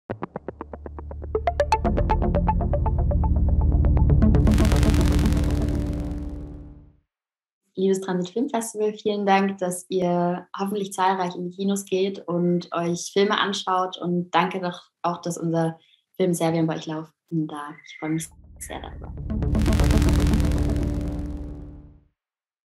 Maria Dragus (SERVIAM) | Videobotschaft | Transit Filmfest
Transit Filmfest Regensburg – IT'S A LIMINAL WORLD! | 23.–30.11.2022
SERVIAM – ICH WILL DIENEN (Ruth Mader, AUT 2022)
Pssst! Zutritt streng verboten! Ruth Maders maximale Abrechnung mit katholischem Fanatismus